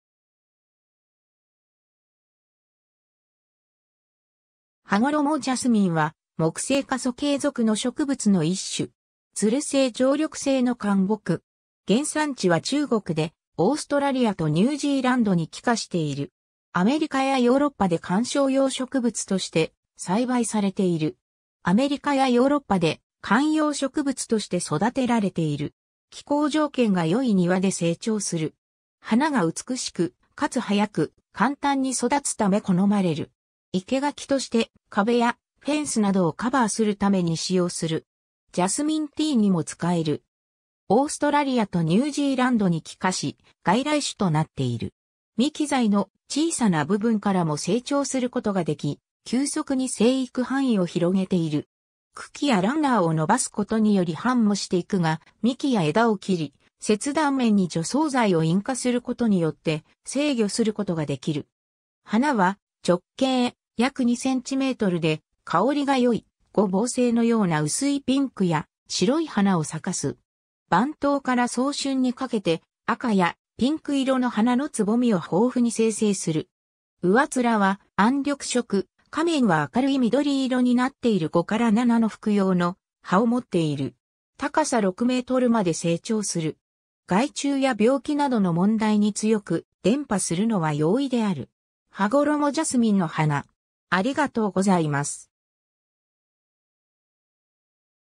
ハゴロモジャスミン
ハゴロモジャスミン（羽衣素馨、英:Pink Jasmine、Jasminum polyanthum）は、モクセイ科ソケイ属（素馨属 Jasminum）の植物の一種。
蔓性常緑性の灌木。
 原産地は中国で、オーストラリアとニュージーランドに帰化している。
アメリカやヨーロッパで観賞用植物として栽培されている。
 アメリカやヨーロッパで観葉植物として育てられている。
気候条件が良い庭で生長する。
 花が美しく、かつ早く、簡単に育つため好まれる。
生垣として壁やフェンスなどをカバーするために使用する。
 ジャスミンティーにも使える。
 オーストラリアとニュージーランドに帰化し、外来種となっている。
幹材の小さな部分からも成長することができ、急速に生育範囲を広げている。
茎やランナーを伸ばすことにより繁茂していくが、幹や枝を切り、切断面に除草剤を印加することによって制御することができる。
 花は直径約2 cmで香りがよい、五芒星のような薄いピンクや白い花を咲かす。
晩冬から早春にかけて赤やピンク色の花のつぼみを豊富に生成する。
上面は暗緑色、下面は明るい緑色になっている5〜7の複葉の葉を持っている。
高さ6 mまで生長する。
害虫や病気などの問題に強く、伝播するのは容易である。
 ハゴロモジャスミンの花